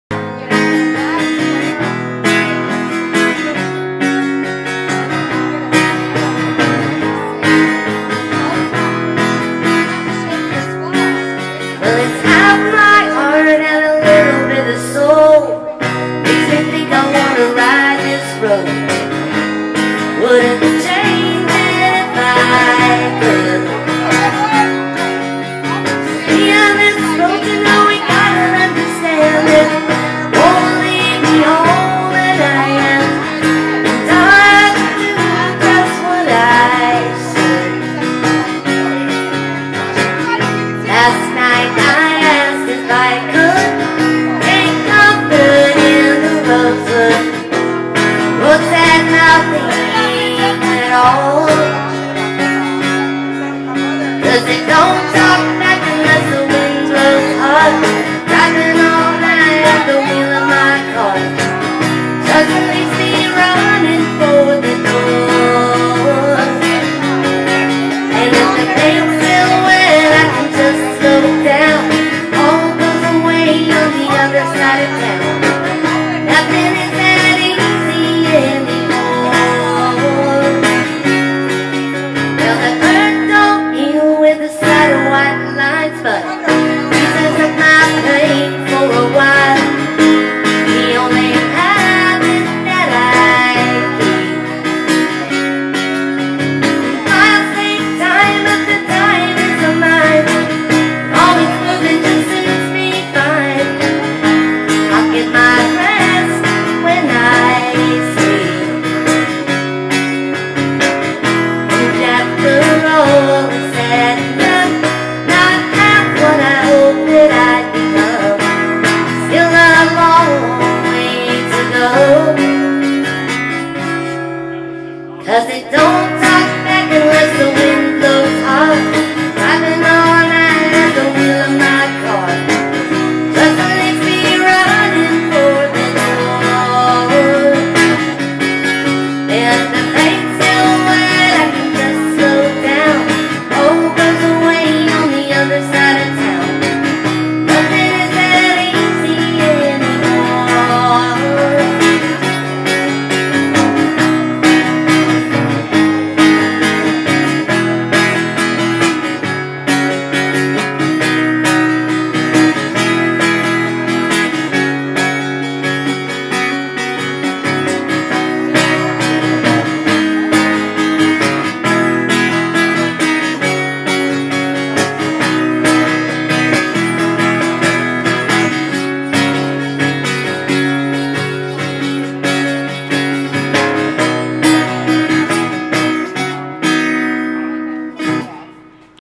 Zoe playing Sabine Sandbar aka Dale's Big Deck, Pensacola Beach around 2011?